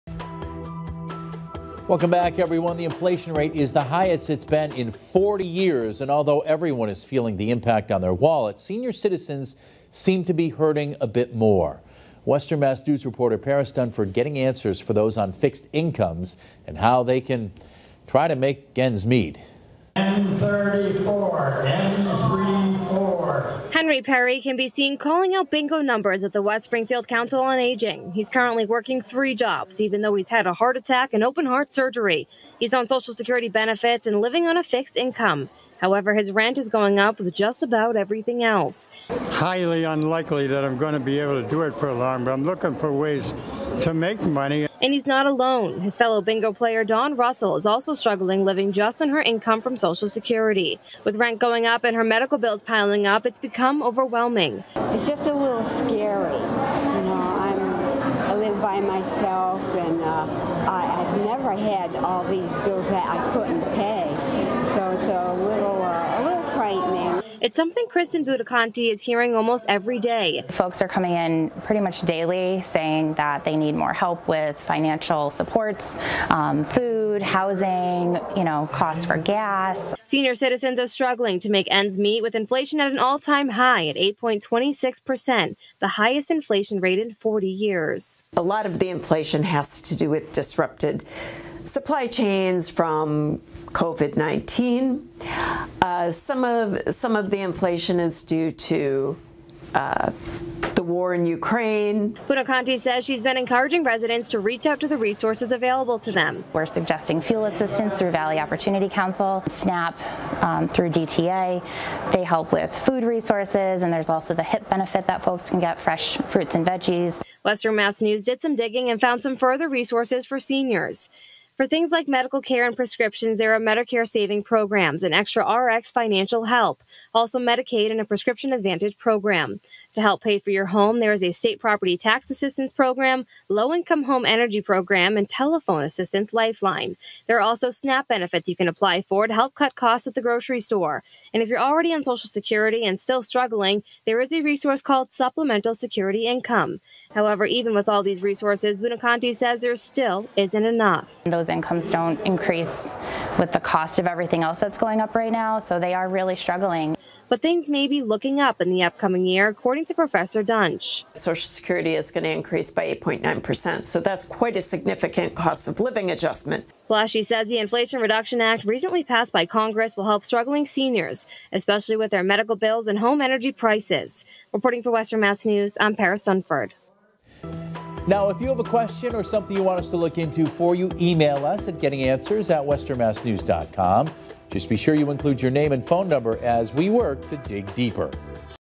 Getting Answers: what resources are available to help seniors with inflation?
Western Mass News is getting answers for those on fixed incomes and how they can make ends meet.